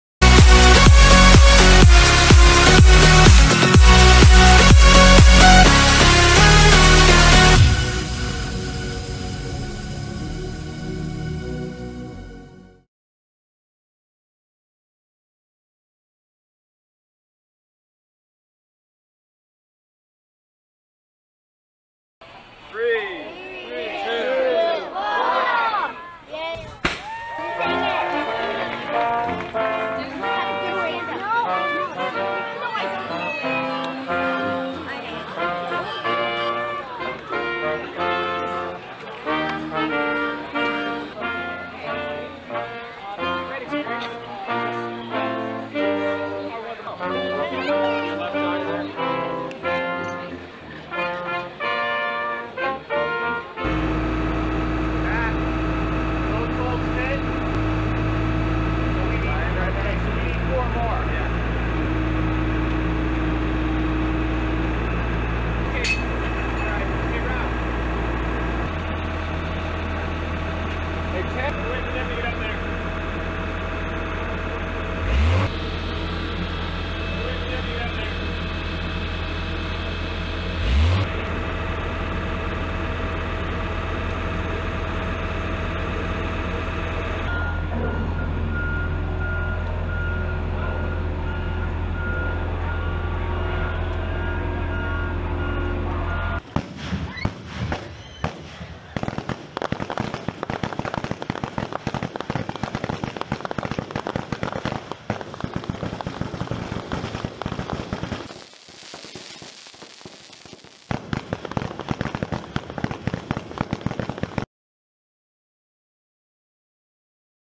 Legoland
Legoland viert 4 juli met nieuw pronkstuk